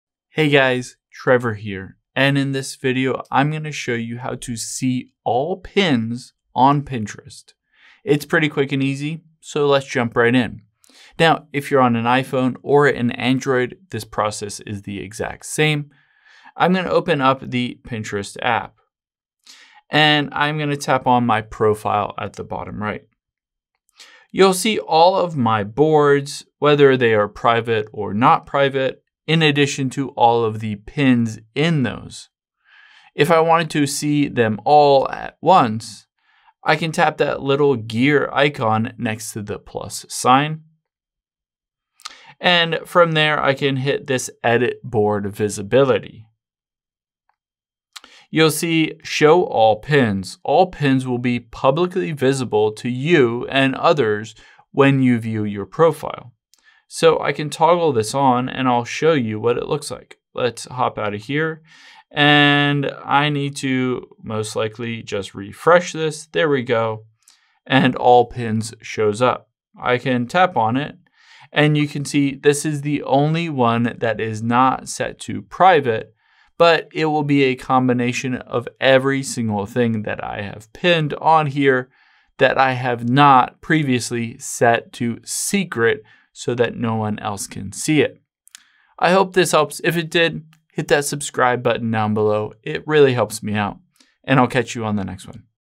How To See All Pins On Pinterest
Here's how you can see all of your pins on Pinterest in one place on your phone or computer.

Cool iPhone Facts:
- Every single iPhone ad shows the time as exactly 9:41